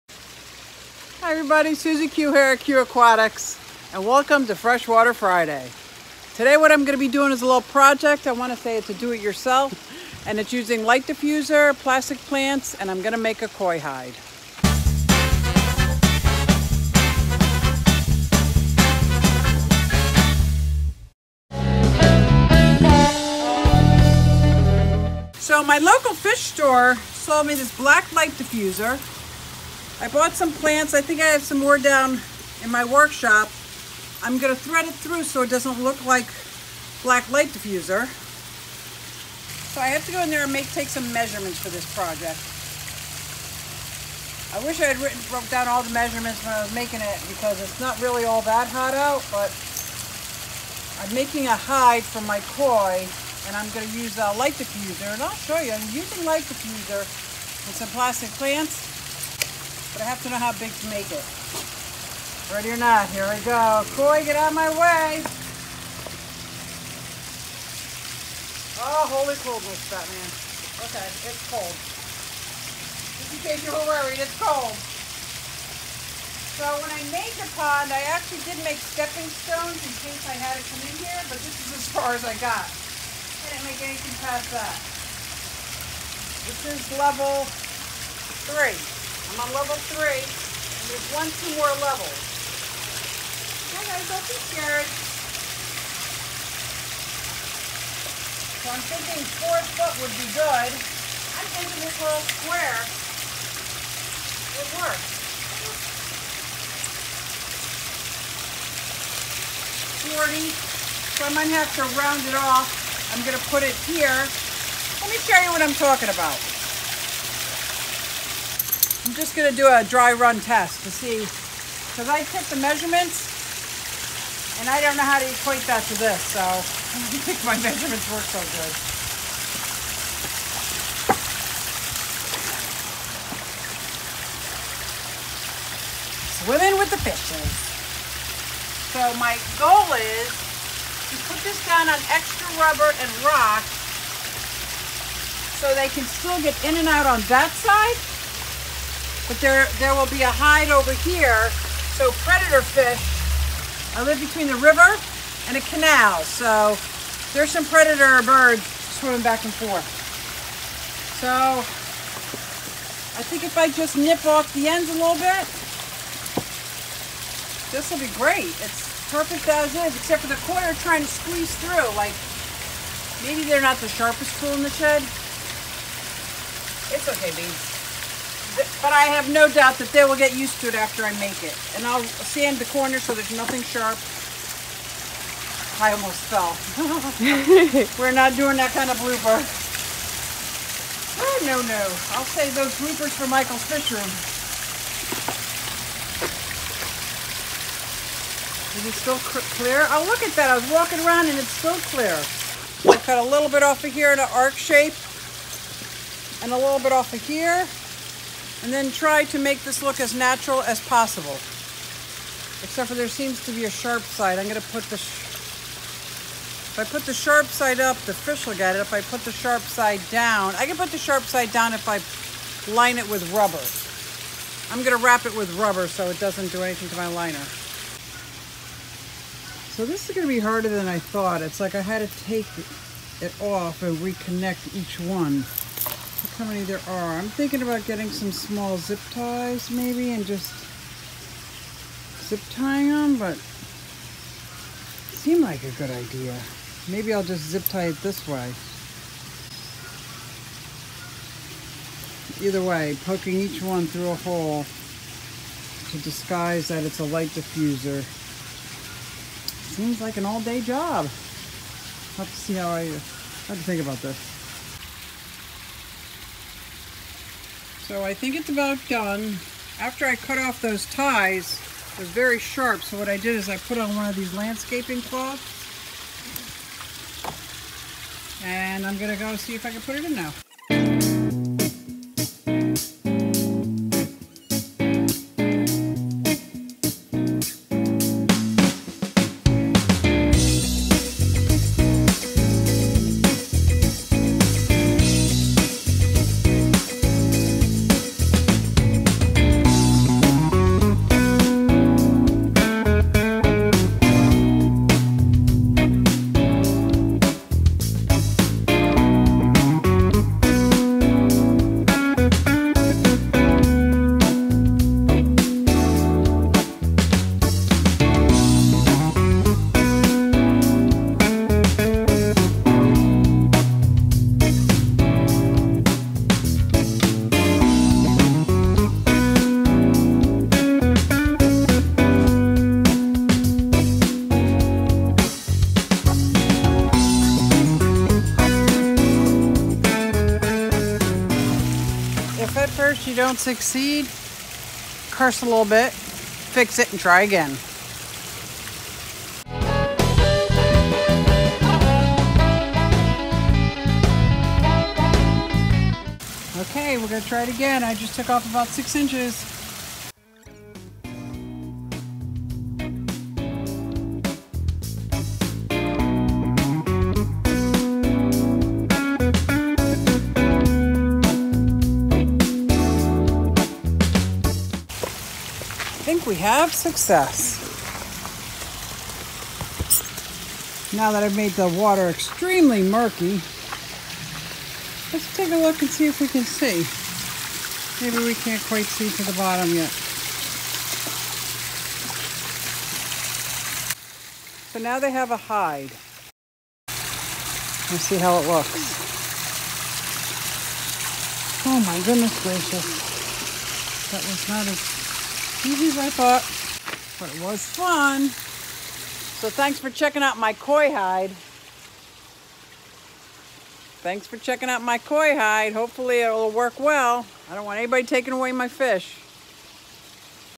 Here is my DIY Koi Hideaway in Attempts to Keep my Koi Safe from Predators
DIY Koi Hideaway for my backyard pond. I live between a canal and a river and I want to make every effort to give my Koi a safe place to hideaway when the predators are around. Using a black light diffuser and some plastic plants, I was able to create something that I hope works.